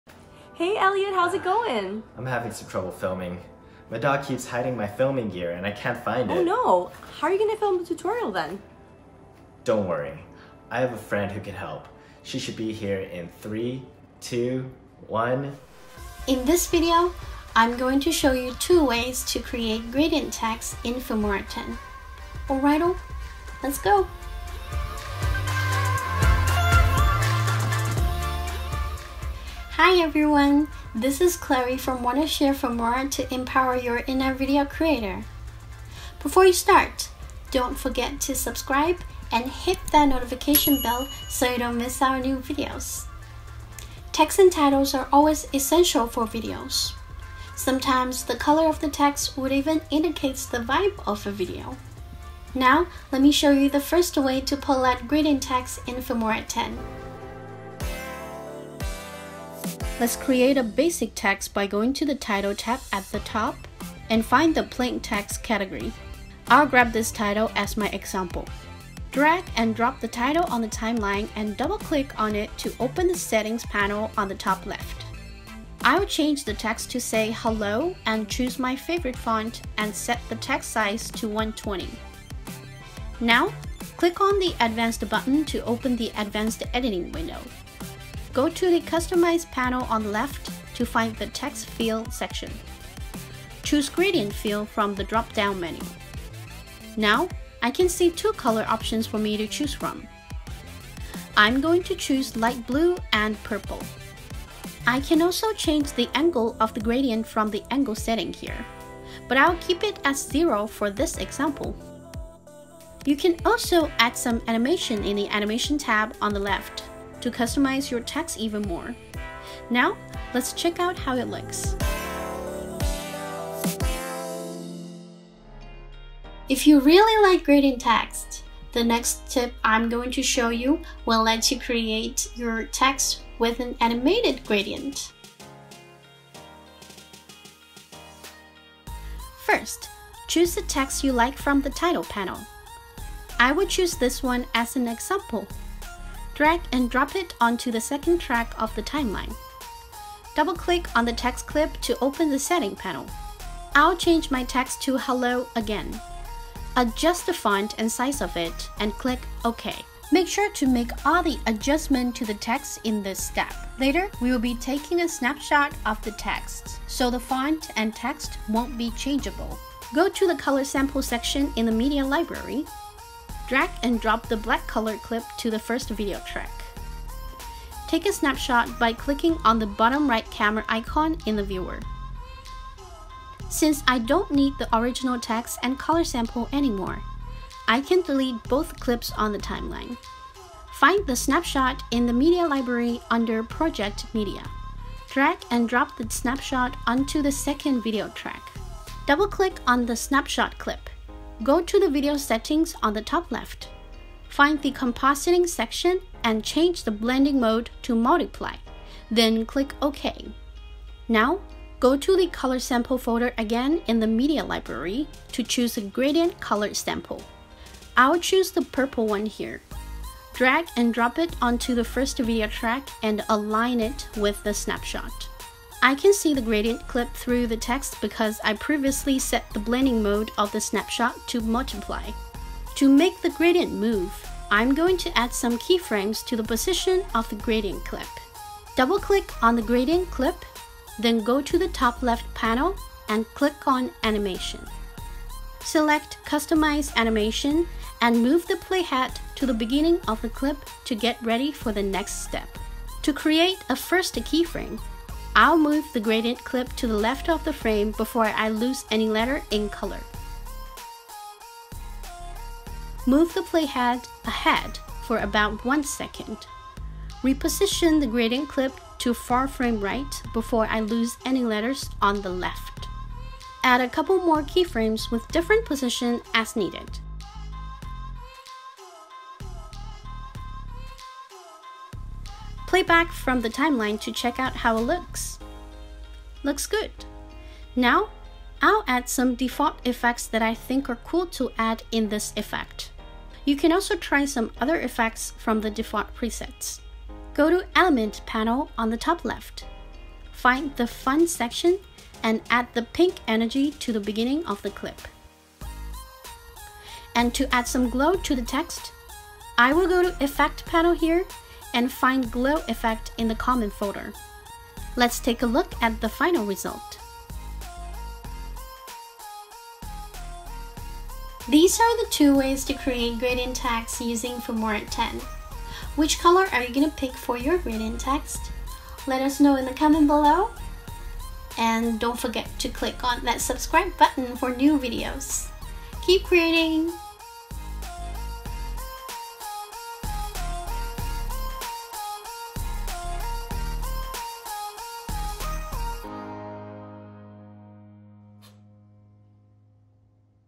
EASY Gradient Text Effect | Wondershare Filmora X Tutorial
Add gradient text effects to your video intros with FilmoraX // Video text and titles are essential for all videos. The color of texts can indicate the vibe of a video easily and also add your unique personality to your videos. There are many easy ways to add text to videos in Wondershare Filmora X and in this video tutorial, our new friend Chloe is going to show you how to create your own customized gradient text effect AND how you can animate the gradient in the text title with keyframes!

0:00 Oh no! Elliot what are you going to do?
0:58 Gradient Text in Wondershare Filmora X
2:16 Animating the gradient in text
5:41 He'll never tell where the gear is.

Always have the latest version! Wondershare Filmora X Download

16-35 Lens
24-70 Lens